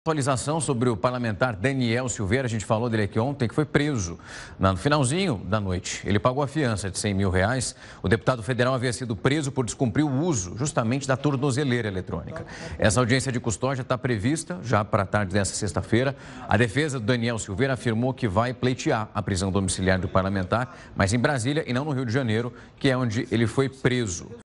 Após ser preso, Daniel Silveira paga fiança de R$ 100 mil
Preso por descumprir o uso da tornozeleira eletrônica, o deputado federal Daniel Silveira pagou, nesta sexta-feira (25), uma fiança de R$ 100 mil.